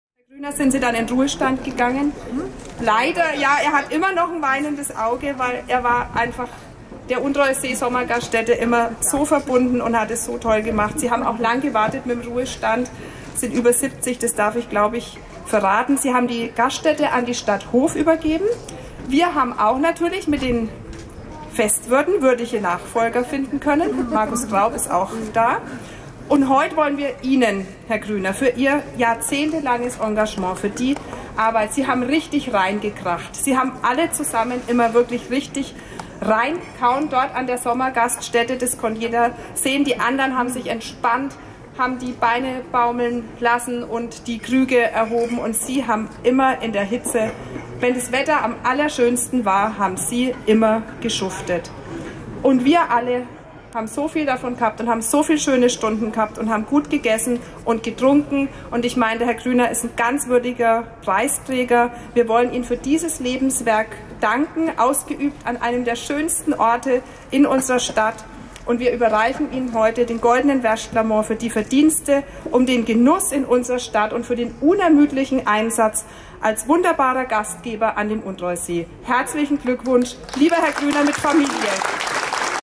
[2160p60] 152 Jahre Wärschtlamo: Der Preis "Der Goldene Wärschtlamo 2023" wird verliehen
Am 06. September 2023 wurde der 152. Jahrestag der Hofer Wärschtlamänner gefeiert. Hierbei wurden die Bürger eingeladen, Musik und Reden zu lauschen und sich an Bier- und Wärschtlaständen verköstigen zu lassen. Auch wurde der goldene Wärschtlamo 2023 verliehen. In diesem Video stellt die Oberbürgermeisterin den Preis "Der Goldene Wärschtlamo 2023" vor. Ihn erhält diejenige Person, die sich jahrzehntelang in der Hofer Kulinarik engagiert hat und damit verdient gemacht hat. Hier sieht man die Laudatio für den Preisträger. Viel Spaß!